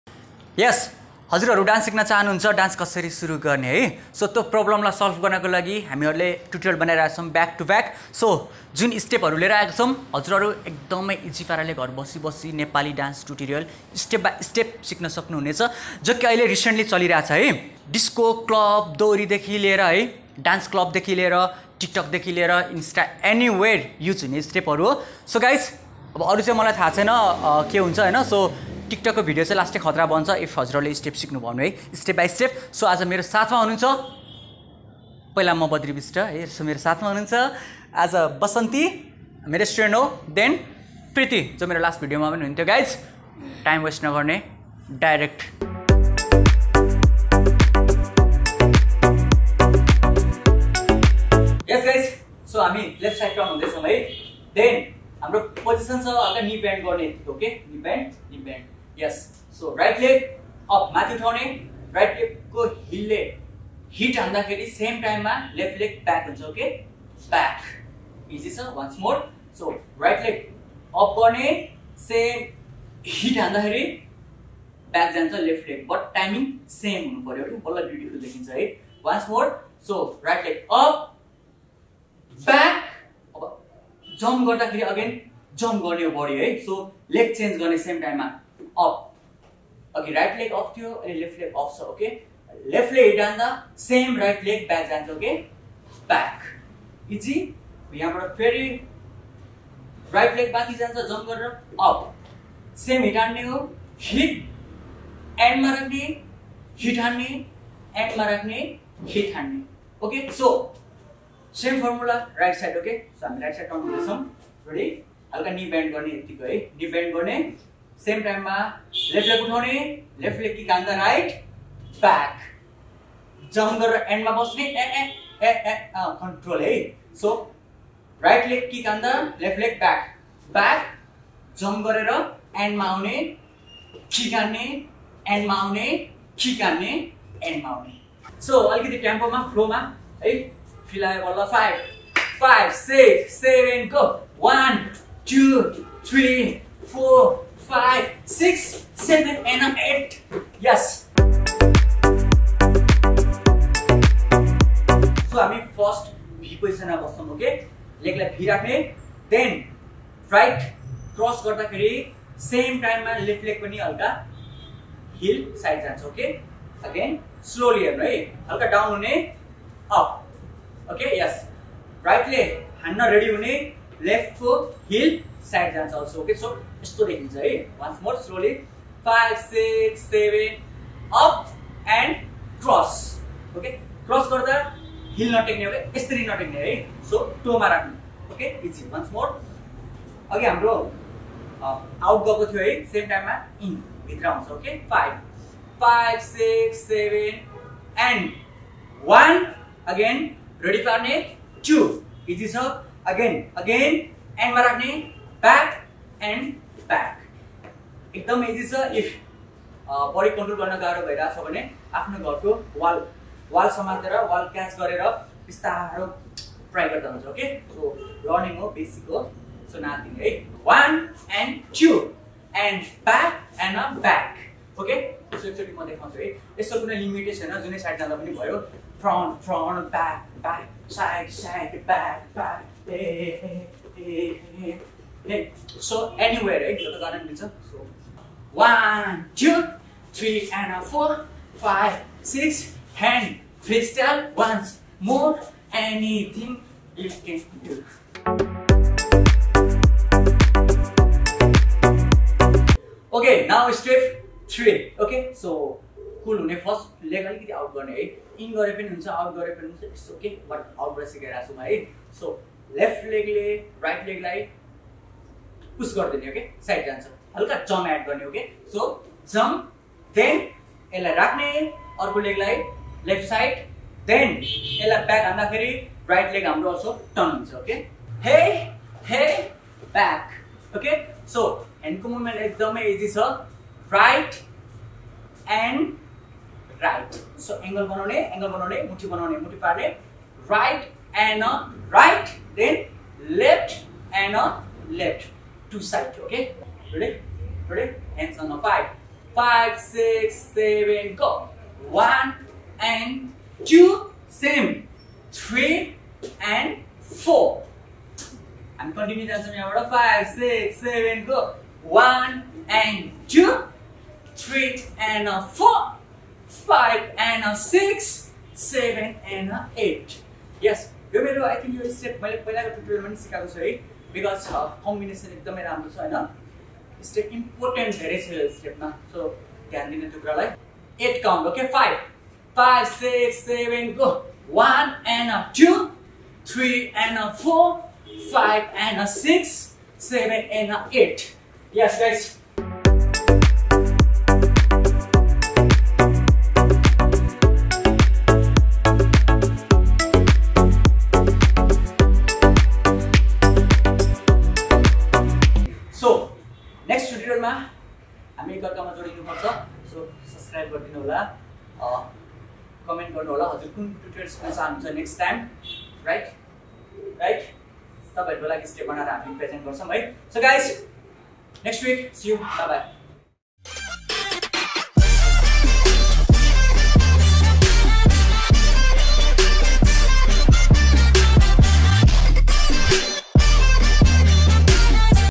( डांस कोर्स EP -13) Basic Nepali Dance Tutorial | Dance Step For Everyone | Badri Bista
Best Nepali Dance Tutorial ( डांस कोर्स  EP -13) Basic Dance Step For Everyone

Thanks To : Preeti / Basanti
Camera/Edit : Prabesh Rai

Online Dance Classes for Everyone Nepali dance tutorial - by Badri Bista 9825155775

location : NepDudes Dance Class Gongabu

Thanks: Kamal Bhakta